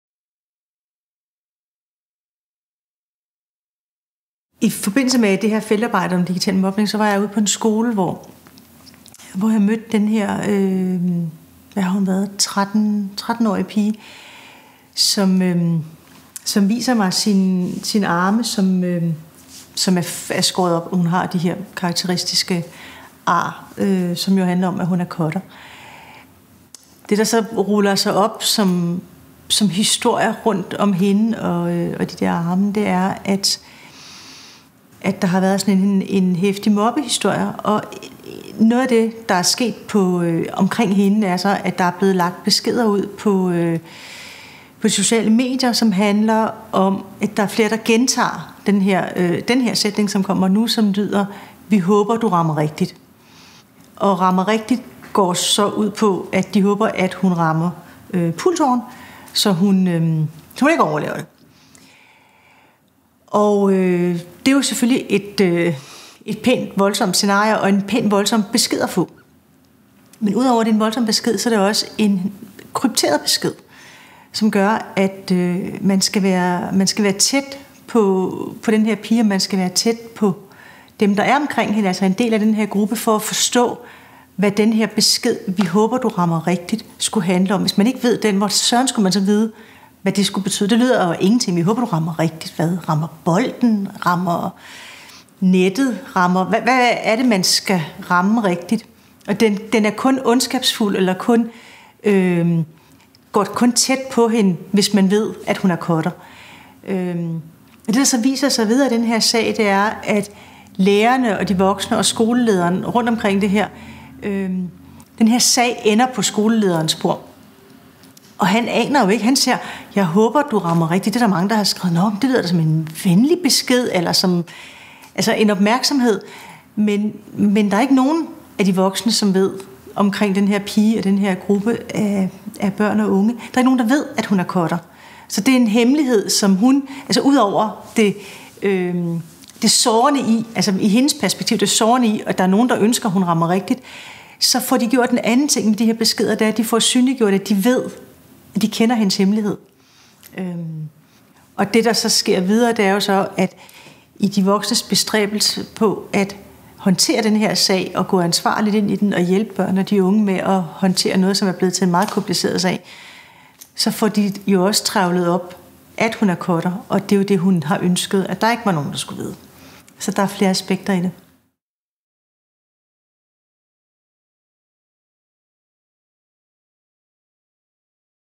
Mobning_042_Online mobning af en cutter
EN trettenårig pige er cutter i det skjulte. Hendes skolekammerater sender hende kodede beskeder: "Vi håber, du rammer rigtigt". Det viser hvor skjult, kodet og svært håndterligt online mobning kan foregå, og hvor svært det er for voksne at forstå det. Jette Kofoed viser hvorfor det er vigtigt at gøre en indsats for at forstå og modvirke online mobning.